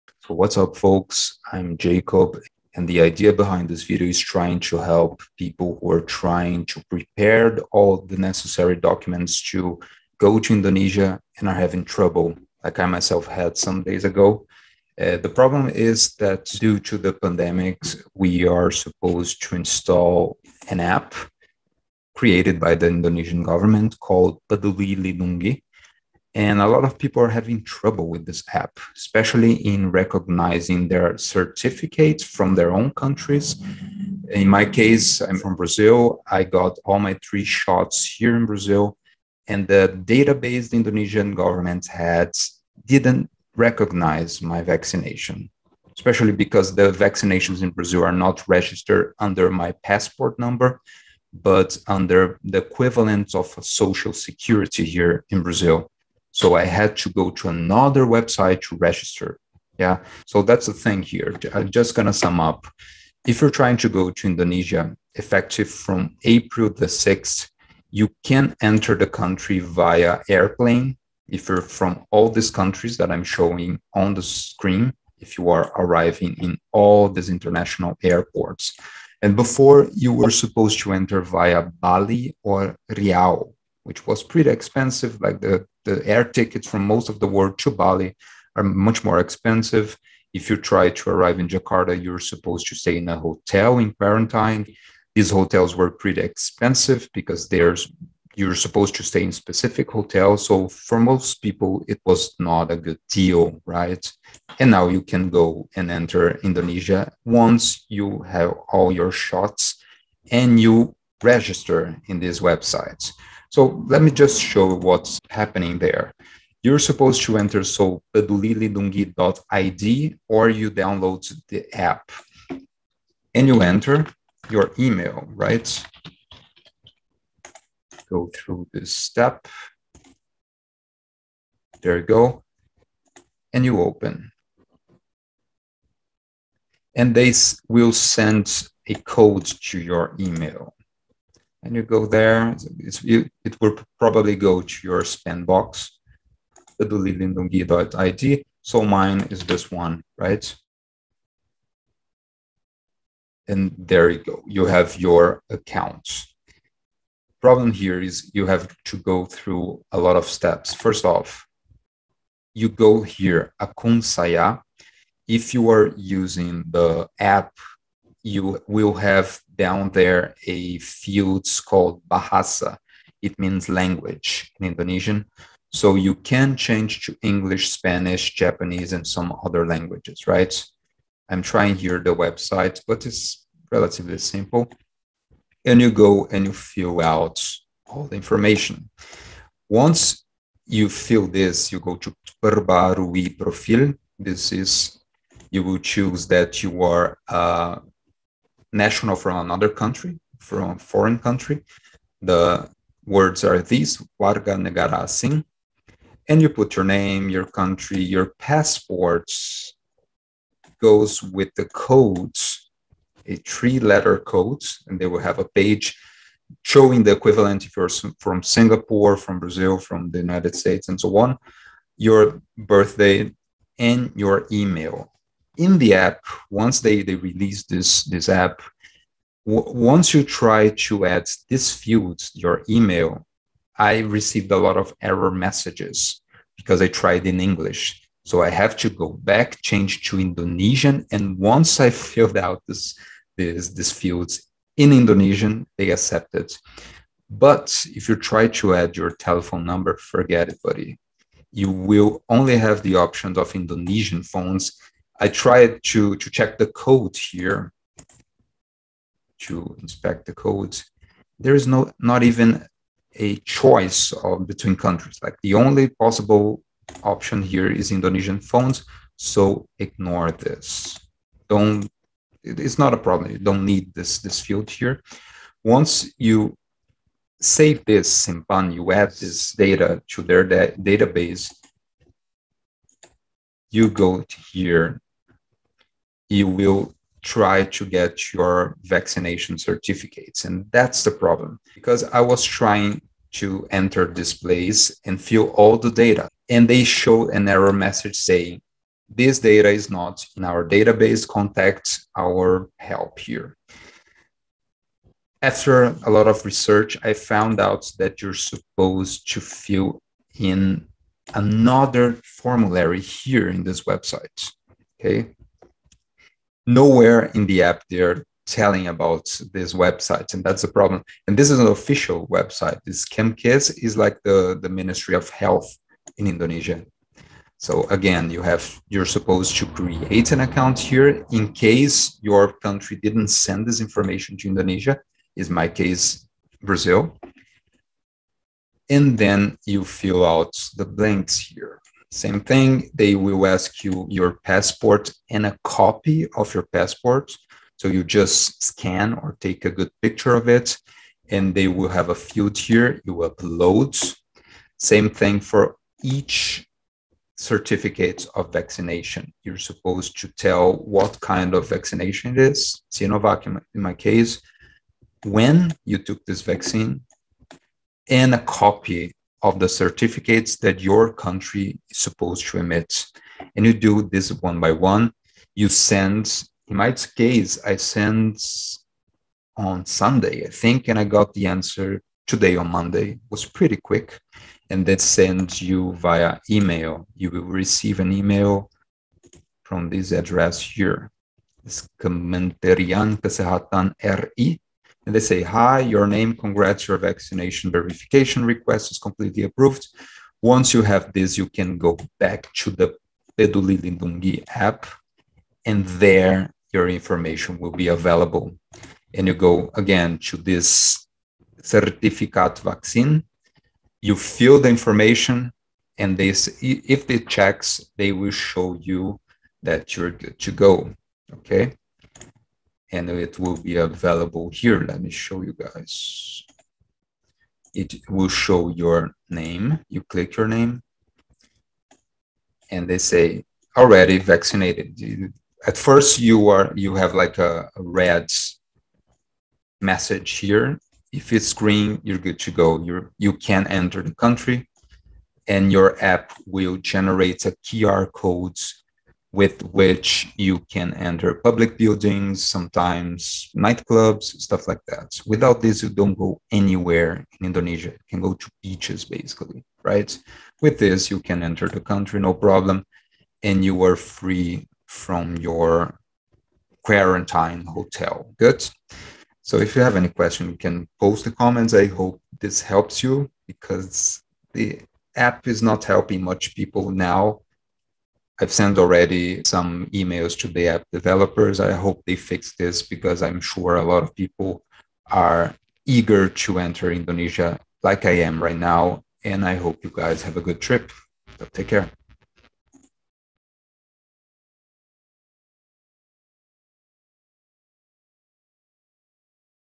Problems with PeduliLindungi App? Here are some solutions (Travel to Indonesia after 06/April/2022)
Here are some tips for those travelers who are trying to validate their vaccination certificates on the Indonesia government APP and are having a hard time.
(2) Click on "Akun saya/My Account" and fill out the whole thing. Ignore the "Nomor telepon" field;
(4) Go back to PeduliLindungi APP or website and click "Sertifikat Vaksin". Fill out the data on each shot you've gotten and validate.
(5) Check whether you have a "green status" (sudah vaksin) on "Vaccination Status".
(6) Have a good trip.